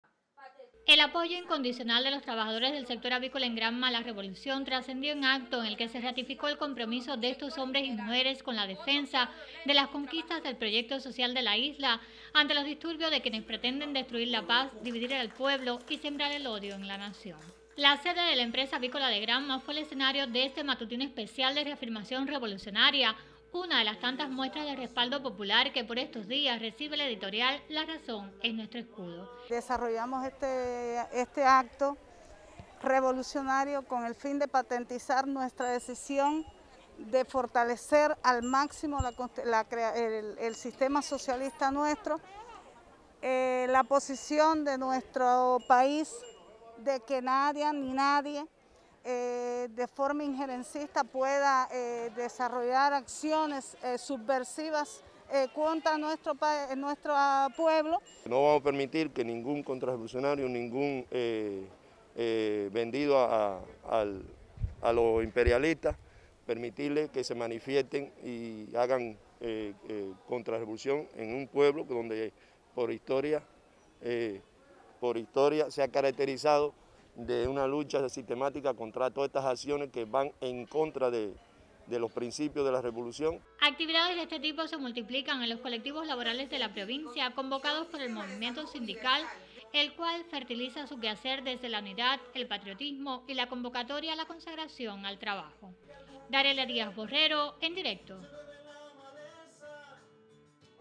Trabajadores del sector avícola reafirman apoyo a la Revolución
Ante los disturbios de quienes pretenden destruir las conquistas de la nación, los trabajadores de Granma convocados por el movimiento sindical expresan su apoyo incondicional a la Revolución y ratifican el compromiso de los colectivos laborales de elevar la eficiencia en la producción de bienes y servicios. Uno de esos espacios de reafirmación revolucionaria tuvo lugar en la sede de la Empresa Avícola de Granma.